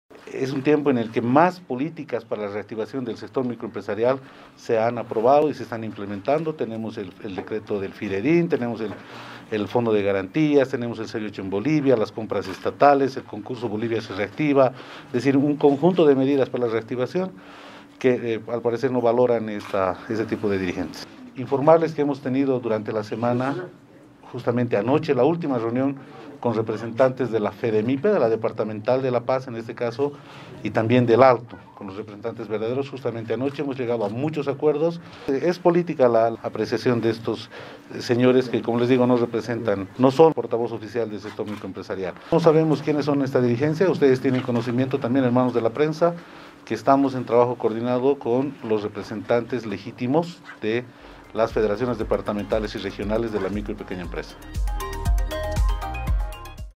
Reactivación Productiva
🎯 ReactivaciónProductiva || El viceministro de la Micro y Pequeña Empresa, Nelson Aruquipa, afirmó que el Gobierno nacional, desde la recuperación de la democracia en Bolivia en noviembre de 2020, implementa varias políticas para la reactivación y apoyo al sector micro y pequeño empresario del país.